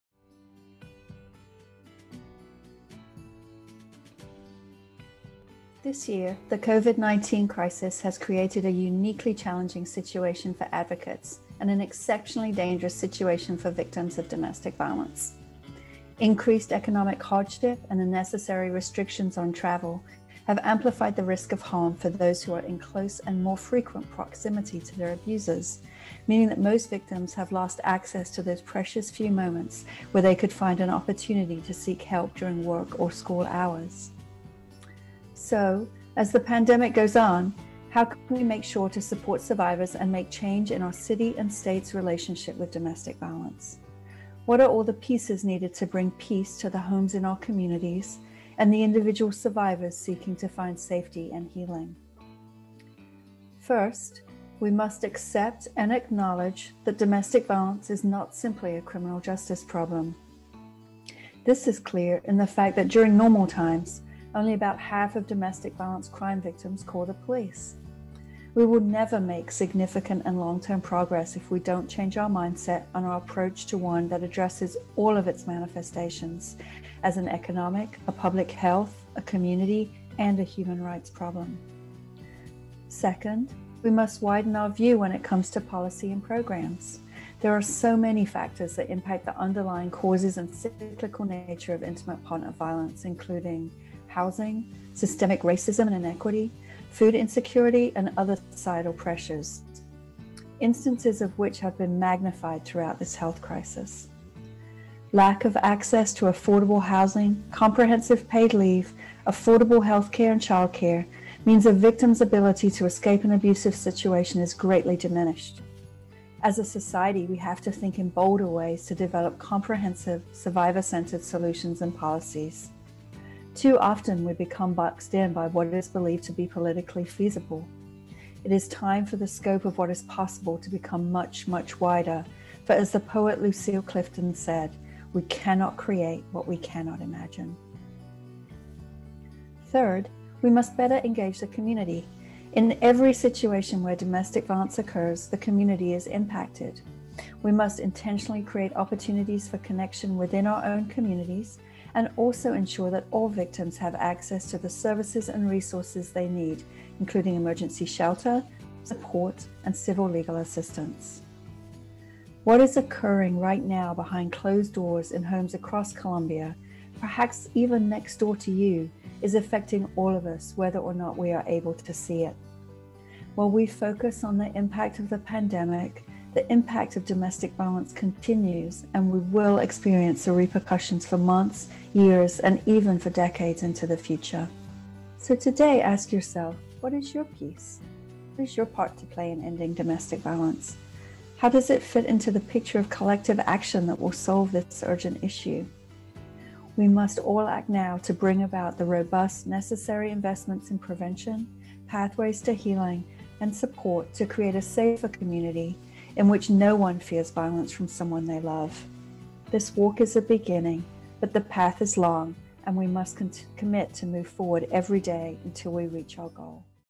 2020 Mayor's Walk Against Domestic Violence | Sara Barber
Thank you for raising awareness for domestic violence and supporting victims of domestic violence in our community!

Sara Barber is the Executive Director of the South Carolina Coalition Against Domestic Violence and Sexual Assault (SCCADVASA).